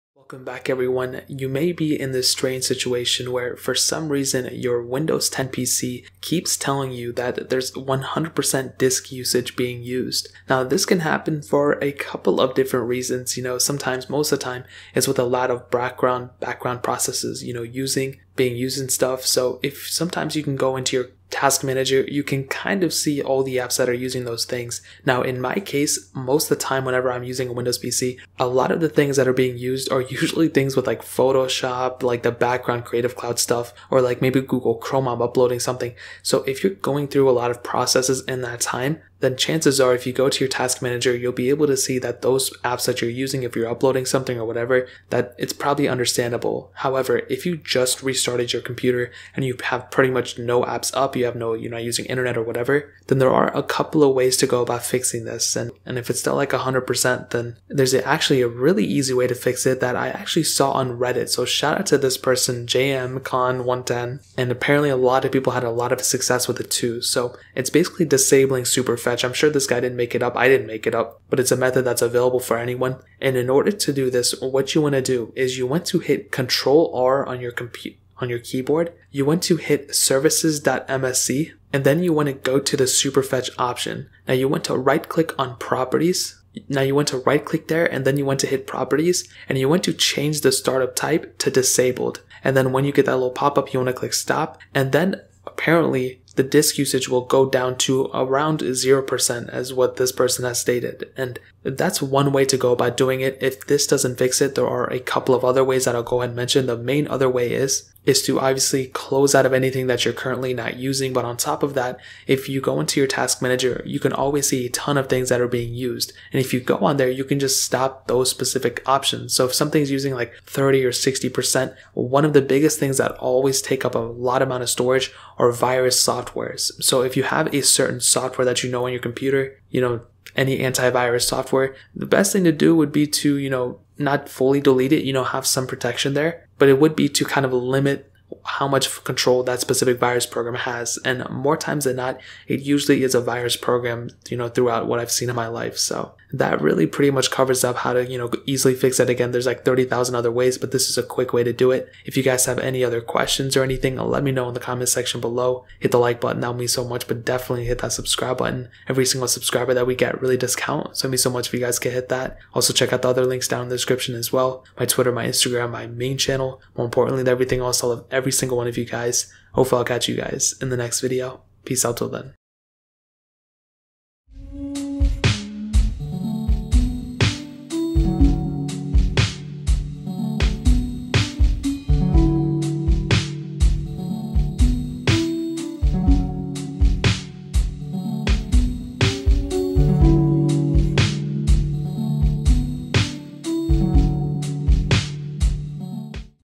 How To Fix 100% Disk Usage On Windows 10!
Here is exactly how to fix your Windows 10 PC when your disk usage is always stuck at 100%!